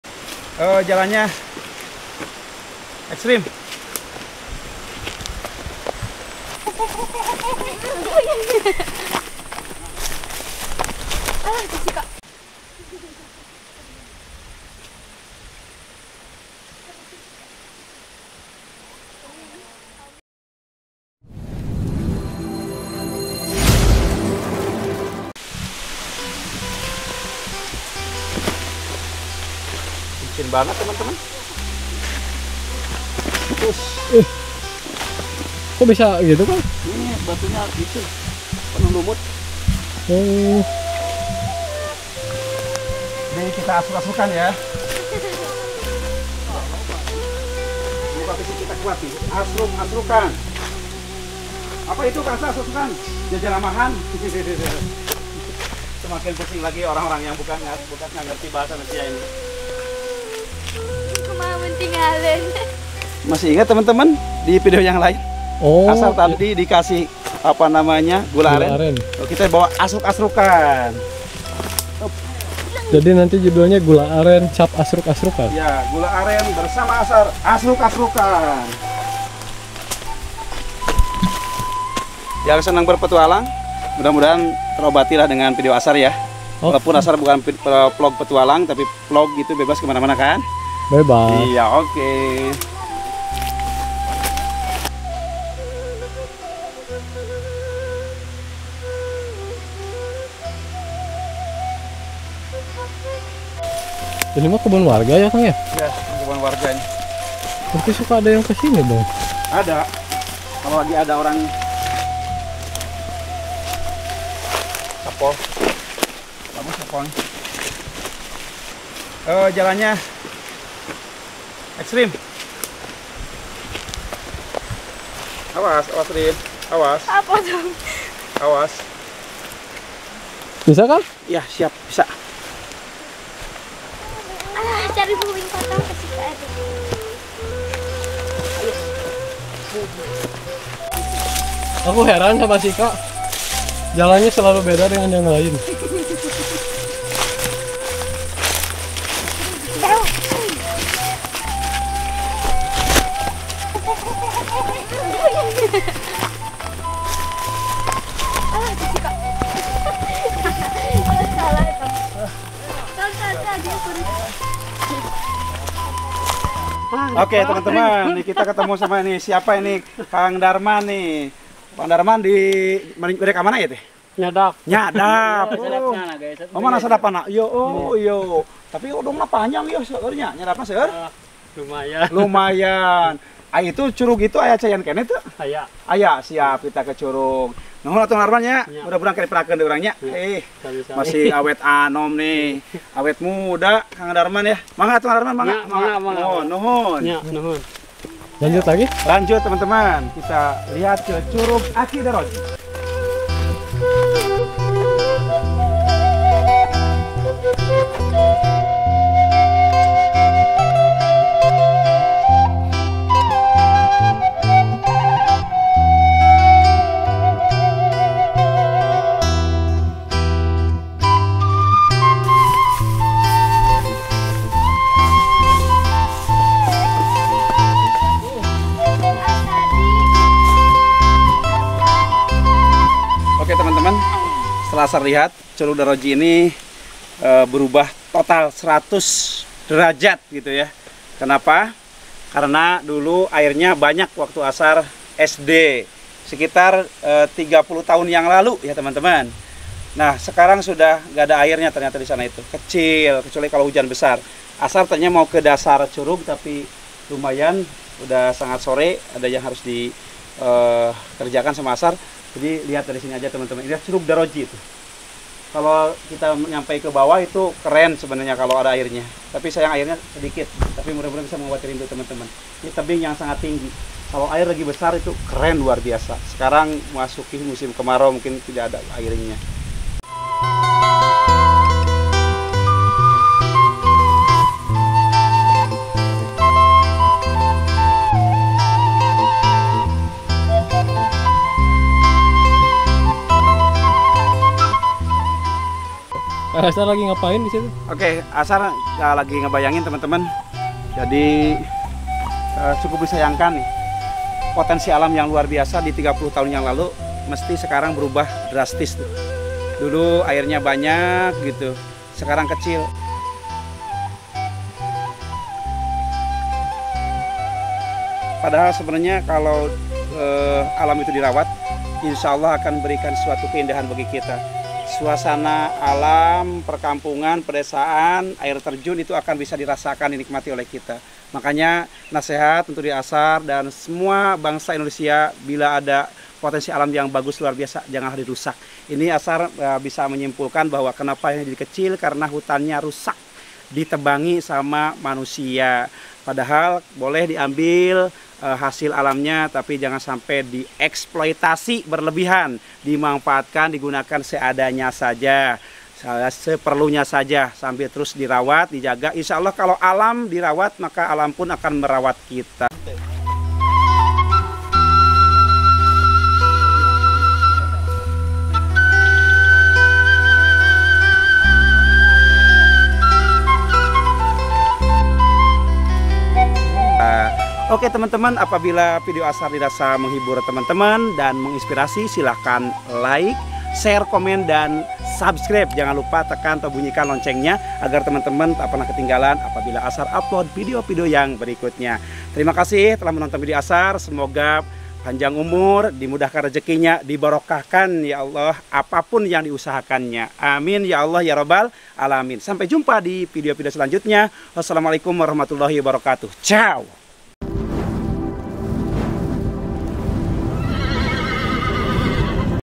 INDAH!!! Pemandangan Alam Cianjur, Suasana Pedesaan Jawa Barat, Beautiful Village Atmosfer
INDAH!!! Pemandangan Alam Cianjur, Suasana Pedesaan Jawa Barat, Beautiful Village Atmosfer I Kang Asar Vlog

Pada Kesempatan Kali Ini Seperti Biasa Asar Akan Berkunjung Ke Salasatu Perkampungan Dan Pedesaan Khususnya Yang Berada Di Indonesia, Untuk Melihat Suasana Langka, Ngangenin Dan Jarang Terlihat! Indahnya Pemandangan Alam Cianjur, Suasana Pedesaan Jawa Barat, Beautiful Village Atmosfer Dengan Suasana Pedesaan Yang Masih Asri, Alami, Sejuk dan Juga Nyaman Yang Berada Di Kawasan Pegunungan.

Asar akan mengunjungi salah satu Curug atau air terjun yang masih alami yang jarang di jumpai oleh masyarakat yaitu Curug Daroji yang berada di pegunungan kampung Pasagi Desa Wanasari Kecamatan Naringgul Cianjur Jawa Barat.
Kampung Asri Nan Indah, Damai, Sejuk dan Juga Nyaman Di Lembah Bawah Bukit.